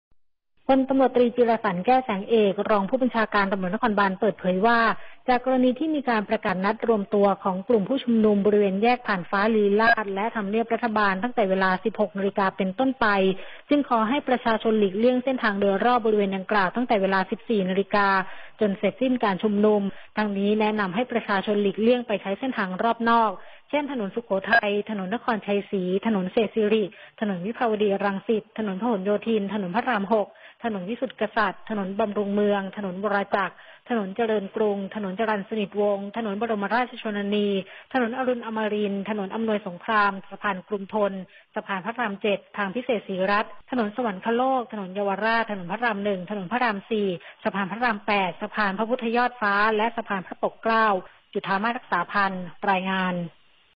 แนะประชาชนหลีกเลี่ยงเส้นทาง บริเวณแยกผ่านฟ้าลีลาศ และทําเนียบรัฐบาล
พลตำรวจตรีจิรสันต์ แก้วแสงเอก รองผู้บัญชาการตำรวจนครบาล เปิดเผยว่า จากกรณีที่มีการประกาศนัดรวมตัวของกลุ่มผู้ชุมนุม บริเวณแยกผ่านฟ้าลีลาศ และทําเนียบรัฐบาล ตั้งแต่เวลา 16.00 น. เป็นต้นไป จึงขอให้ประชาชนหลีกเลี่ยงเส้นทางโดยรอบบริเวณดังกล่าว ตั้งแต่เวลา 14.00 น. เป็นต้นไป จนเสร็จสิ้นการชุมนุม ทั้งนี้ แนะนําให้ประชาชน หลีกเลี่ยงไปใช้เส้นทางรอบนอก เช่น ถนนสุโขทัย ถนนนครไชยศรี ถนนเศรษฐศิริ ถนนวิภาวดีรังสิต ถนนพหลโยธิน ถนนพระราม 6 ถนนวิสุทธิกษัตริย์ ถนนบํารุงเมือง ถนนวรจักร ถนนเจริญกรุง ถนนจรัญสนิทวงศ์ ถนนบรมราชชนนี ถนนอรุณอัมรินทร์ ถนนอํานวยสงคราม สะพานกรุงธน (ซังฮี้) สะพานพระราม 7 ทางพิเศษศรีรัช ถนนสวรรคโลก ถนนเยาวราช ถนนพระราม 1 ถนนพระราม 4 สะพานพระราม 8 สะพานพระพุทธยอดฟ้า และสะพานพระปกเกล้า